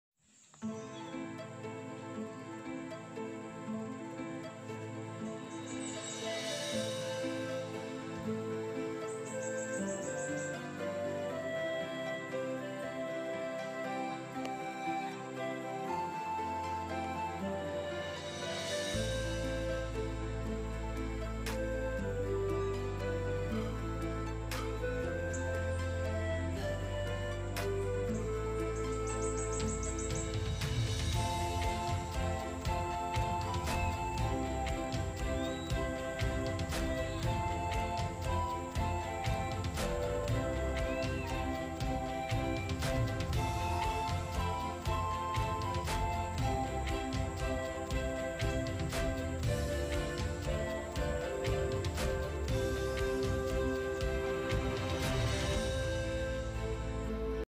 Different type of moths
Hi there ♥️ today i would like to present to you all the different types of moth through this video